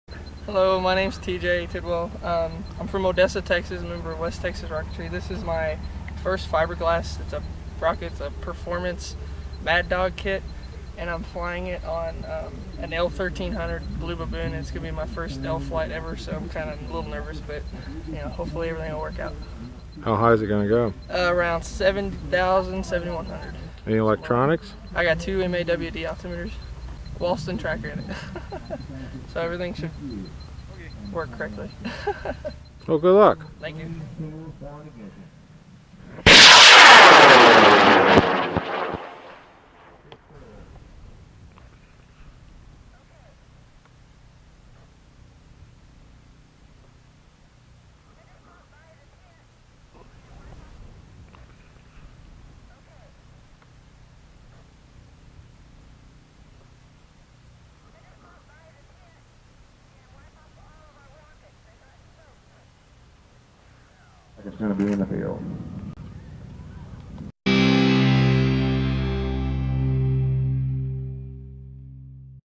TJ Tidwell Mad Dog L1300
Cool Flight of T.J. Tidwell Mad Dog on an L-1300